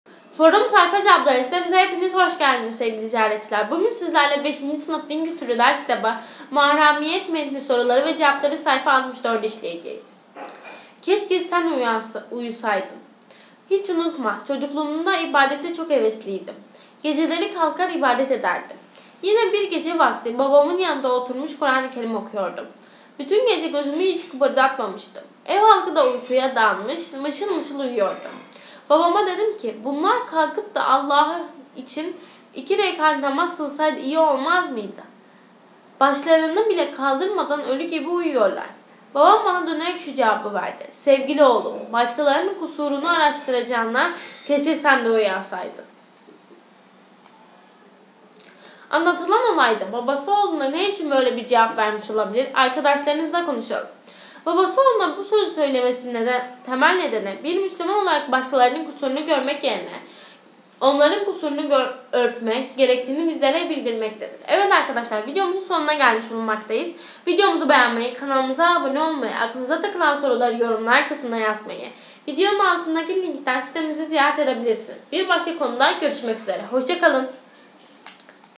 5. Sınıf Din Kültürü ve Ahlak Bilgisi 64. Sayfa Cevapları Lisans Yayıncılık Ders Kitabı
5. Sınıf Din Kültürü Lisans Yayıncılık Ders Kitabı Mahremiyet Metni Soruları ve Cevapları Sayfa 64